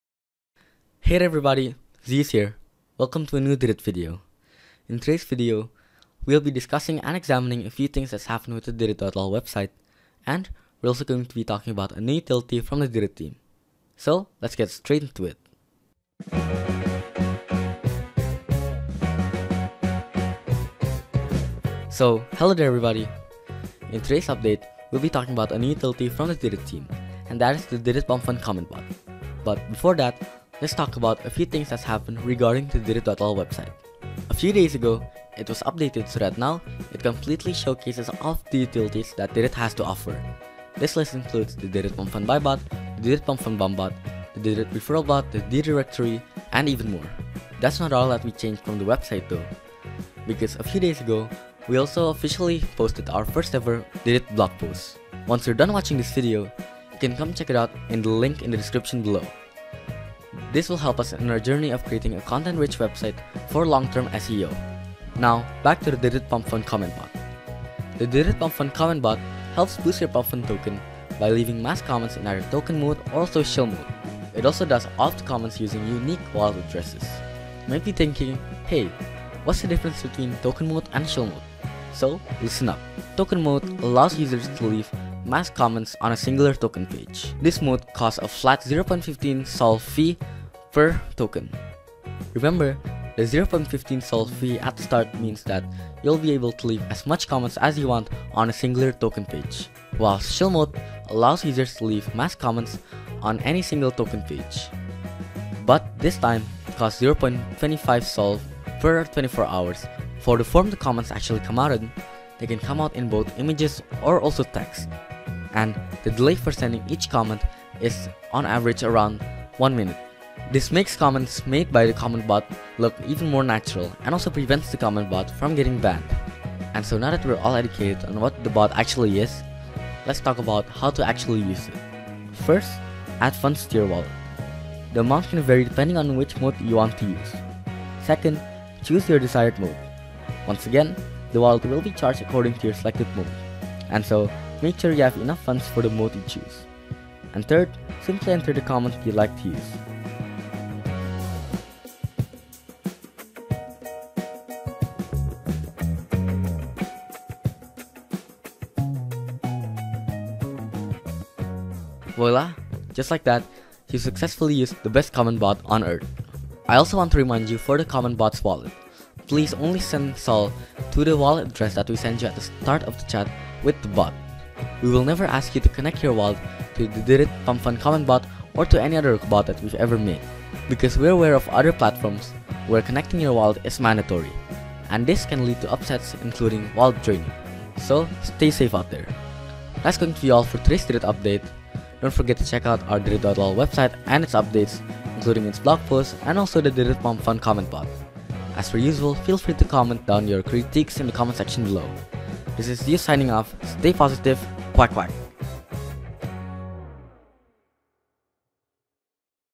Didid Pump.fun Comment Bot - Update #7
This time, we will discuss a very powerful tool from Didid called the Didid Comment Bot, which has so far been used and has helped hundreds of developers on the Pumpfun platform.